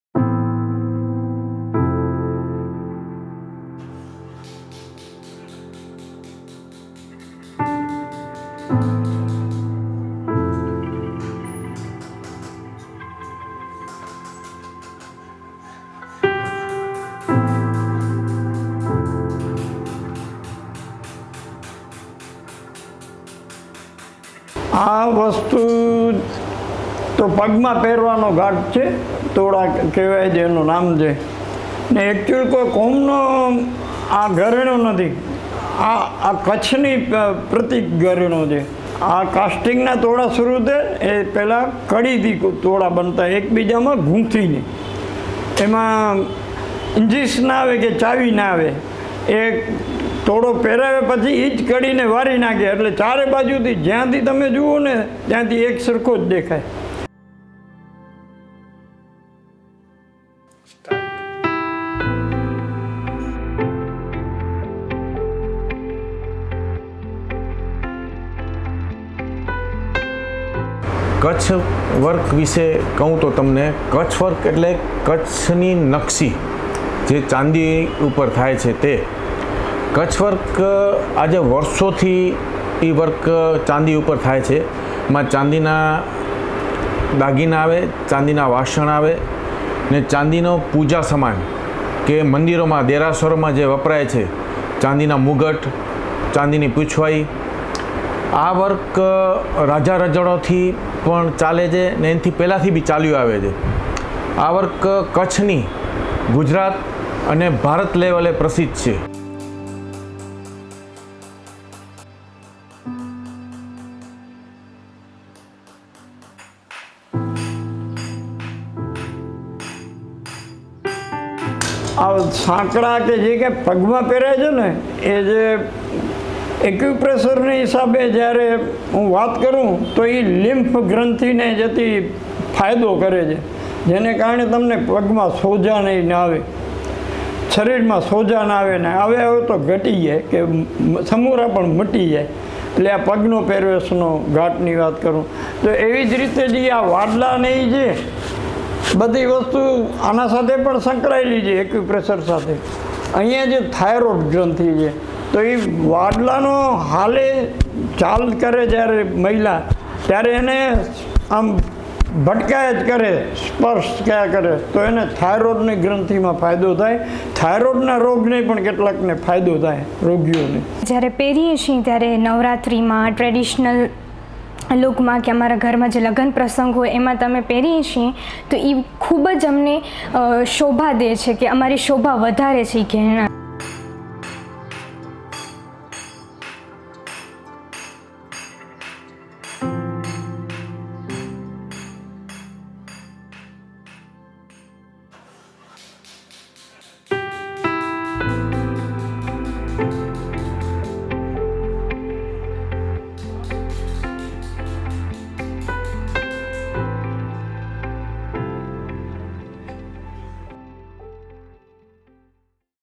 Kutchi Jewellery: Makers claim it has healing powers - BBC URDU
انڈیا کی ریاست گجرات میں کچھ کے زیورات اپنے مخصوص بھاری کام اور نقش کے لیے مشہور ہیں۔ کچھ کے کاریگر مانتے ہیں کہ یہ نہ صرف خوبصورت ہیں بلکہ اس کے صحت کے لیے بھی بہت سے فوائد بھی ہیں۔ اس ویڈیو میں ہم آپ کو ایسے ہی کاریگروں سے ملوا رہے ہیں، جو اس بارے میں آپ کو تفصیلات بتائیں گے۔